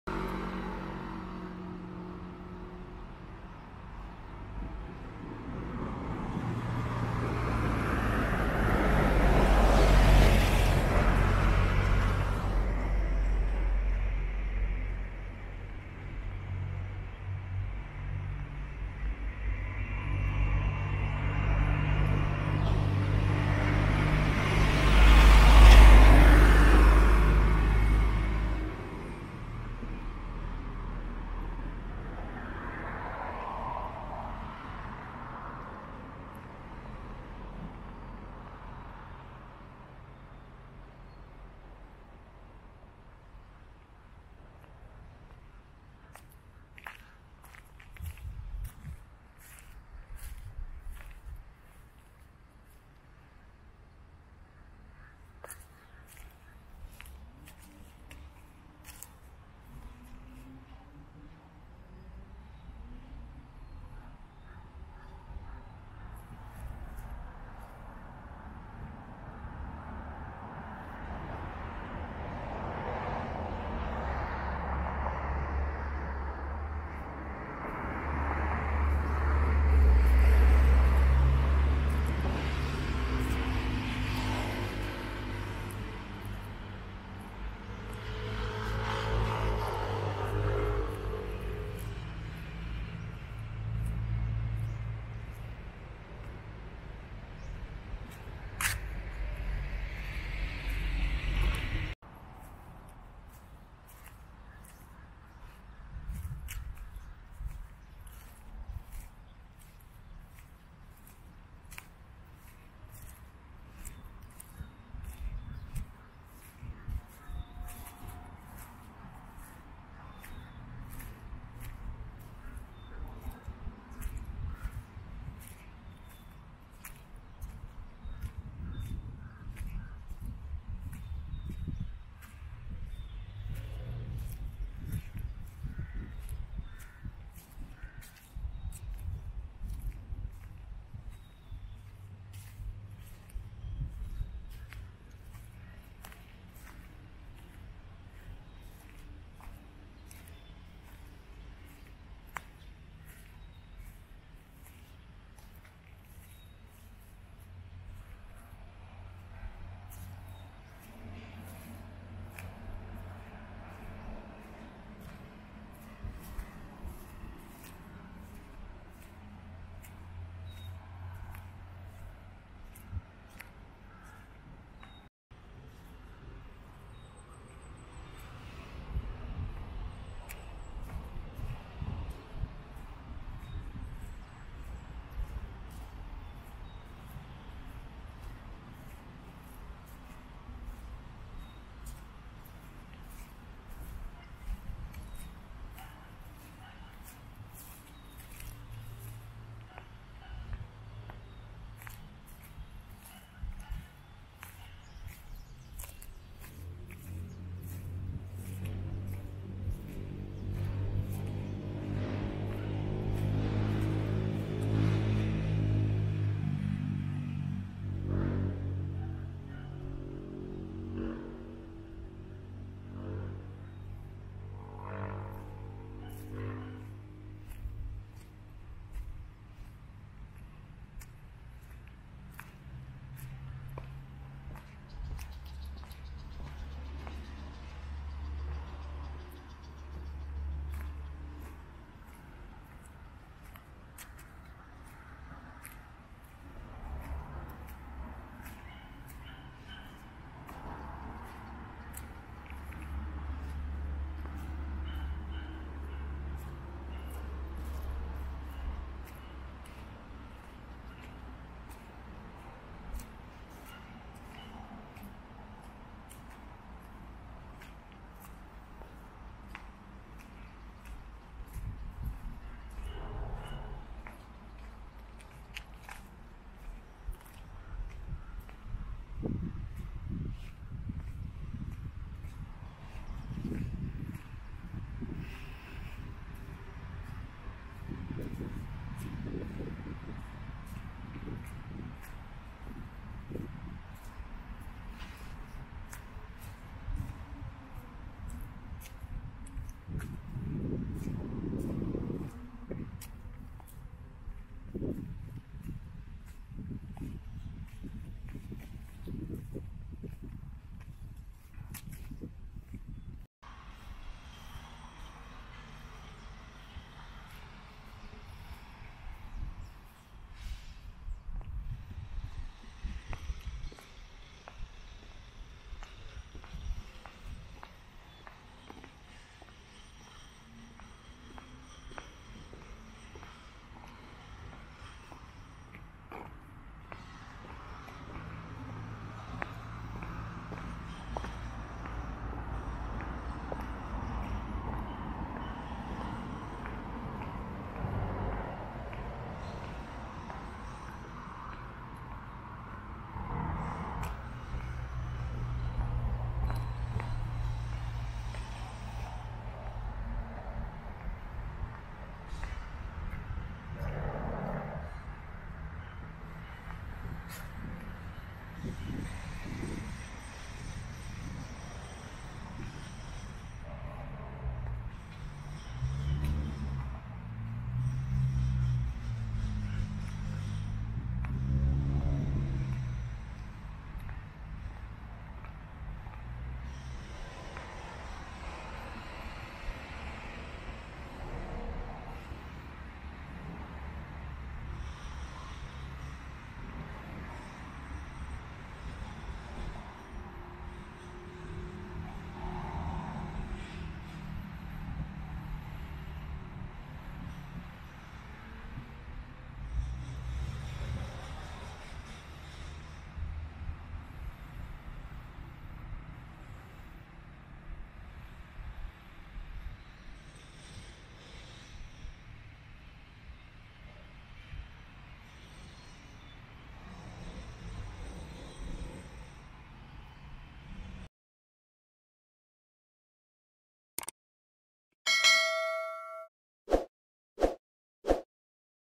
Walking Tour at the Bicentennial Park in Clark Pampanga [4K]
Clark Air Base Bicentennial Park & Recreation Area Walking Tour.

Clark Air Base Bicentennial Park & Recreation Area  is a living monument to the United States of America’s decades-long tenure in Clark, having been opened in 1976 to commemorate the 200th anniversary of American independence from British rule in July 4, 1776. It was renovated by Clark Development Corporation in 2003 and, again, in 2019 to improve and modernize the facilities and amenities within the park, which includes basketball and volleyball courts, playgrounds, and picnic areas. The central Pavilion is available for rent and can accommodate up to 50 - 80 persons. It is the perfect venue for family reunions and gatherings, parties, anniversary celebrations, and other activities.

No talk, just a walking tour video with real ambient sound / pov / asmr from Virtual Trip. For the best viewing experience, watch it in HD or 4K! Enjoy :)

Filmed on: January 15, 2022 / 3:00 PM Philippines Time

Thanks for dropping by! Have a great day and a fantastic life ahead!